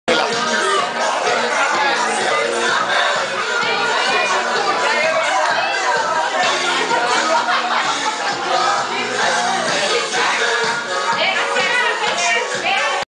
paciente médico e drag queen Tchaka juntos em hospital veterinário.MPG
paciente médico e drag queen Tchaka juntos em hospital veterinário